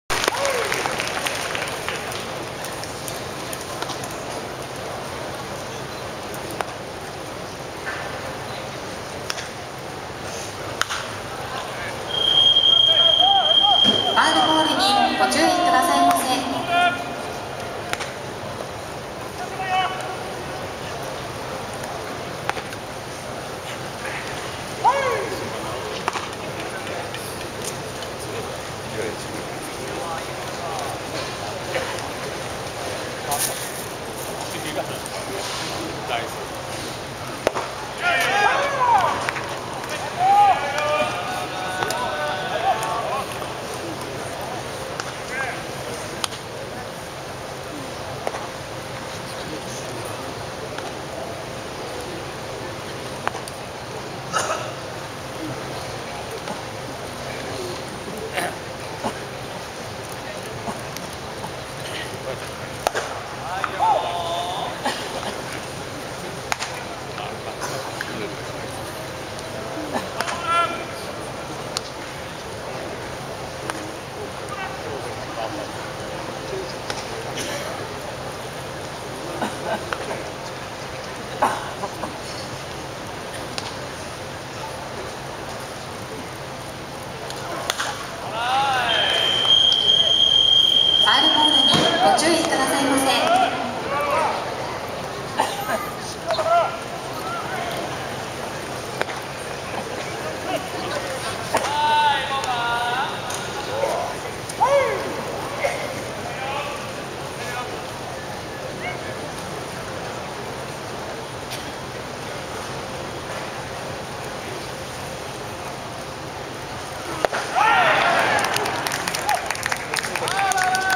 【2023 ウ・リーグ】広島カープ 中村 貴浩のバッティングフォーム_2023060904
2023年ウエスタンリーグ、広島東洋カープ VS オリックスバッファローズ@マツダスタジアムでの広島カープ 中村 貴浩のバッティングフォームです。